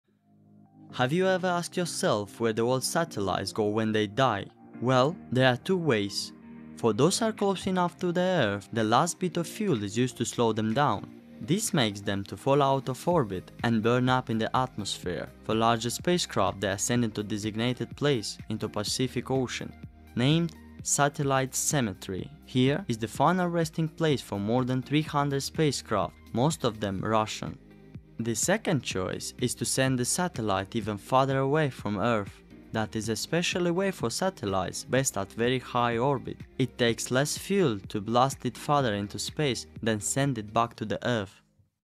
where the old satellite goes when die ?
Two things can happen to old satellites: For the closer satellites, engineers will use its last bit of fuel to slow it down so it will fall out of orbit and burn up in the atmosphere. Further satellites are instead sent even farther away from Earth.

Like every other machine, satellites do not last forever. Whether their job is to observe weather, measure greenhouse gases in the atmosphere, or point away from Earth to study the stars, eventually all satellites grow old, wear out, and die, just like old washing machines and vacuum cleaners.

So what happens when a trusty satellite’s time has come? These days there are two choices, depending on how high the satellite is. For the closer satellites, engineers will use its last bit of fuel to slow it down. That way, it will fall out of orbit and burn up in the atmosphere.

The second choice is to send the satellite even farther away from Earth. It can take a lot of fuel for a satellite to slow down enough to fall back into the atmosphere. That is especially true if a satellite is in a very high orbit. For many of these high satellites, it takes less fuel to blast it farther into space than to send it back to Earth.

Burning metal and "spacecraft cemeteries"
Getting rid of the smaller satellites in low orbits is simple. The heat from the friction of the air burns up the satellite as it falls toward Earth at thousands of miles per hour. Ta-da! No more satellite.

What about bigger things like space stations and larger spacecraft in low orbit? These objects might not entirely burn up before reaching the ground. There is a solution—spacecraft operators can plan for the final destination of their old satellites to make sure that any debris falls into a remote area. This place even has a nickname—the Spacecraft Cemetery! It’s in the Pacific Ocean and is pretty much the farthest place from any human civilization you can find.

What about those higher satellites we blast farther away? Those we send into a “graveyard orbit.” This is an orbit almost 200 miles farther away from Earth than the farthest active satellites. And it’s a whopping 22,400 miles above Earth!

So is that the end of it for these far-away satellites? As far as you and I are concerned it is! However, some of these satellites will remain in orbit for a very, very long time. Perhaps someday in the future, humans may need to send “space garbage trucks” to clean these up. But for now, at least, they will be out of the way.